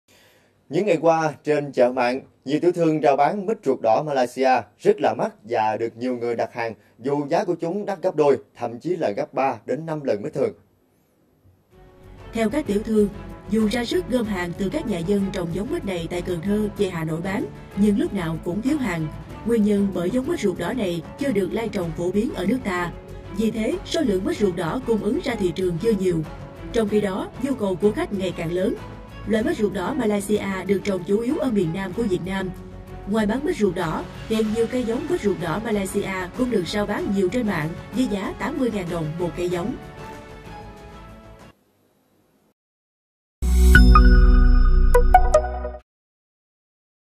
Mít ruột đỏ Malaysia giá 120 000đồng kg vẫn đắt hàng l Nha vuon xuan khuong
Giống Mít Ruột Đỏ Malaysia với màu đỏ đặc trưng, lạ mắt, múi mít dai, vị ngọt thanh ngon hơn mít nghệ truyền thống. Đây chính là một giống mít đặc sản của Malaysia được đưa về Việt Nam trồng thành công trong những năm vừa qua
📌 NHÀ VƯỜN XUÂN KHƯƠNG - XUAN KHUONG GARDEN 📌
🏡 Địa chỉ: Thị trấn Trâu Quỳ, huyện Gia Lâm, TP Hà Nội